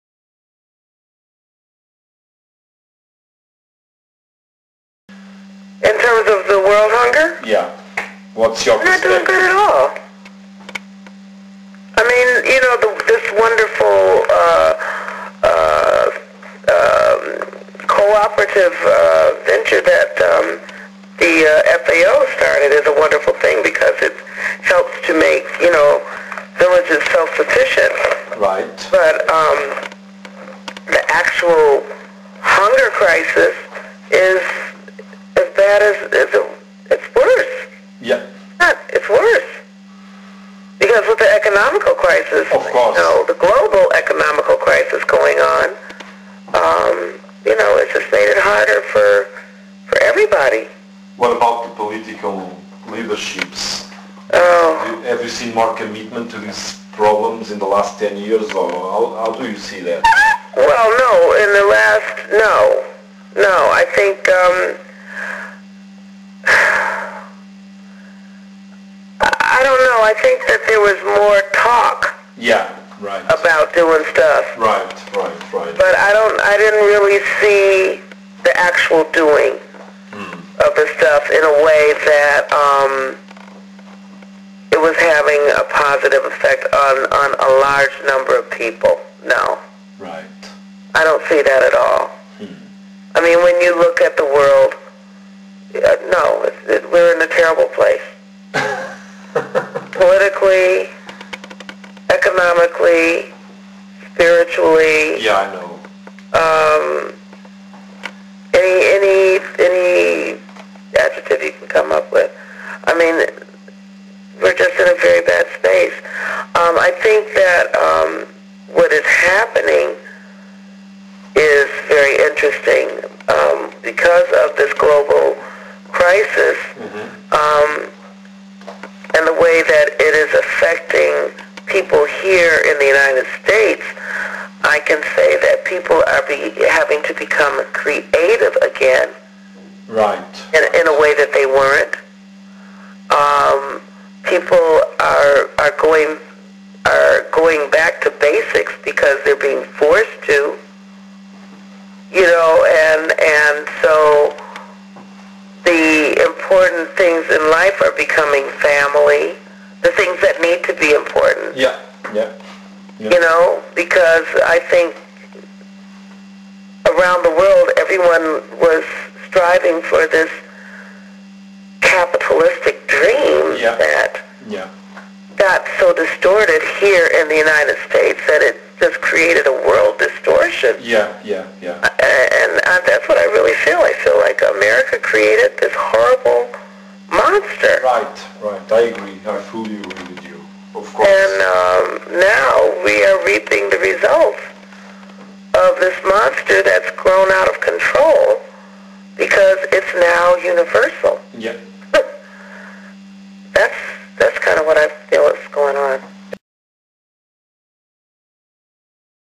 Dee Dee Bridgewater interview (7): Dec. 2009
Dee Dee Bridgewater interviewed by the Portuguese jazz blog Jazz no País do Improviso in December 2009.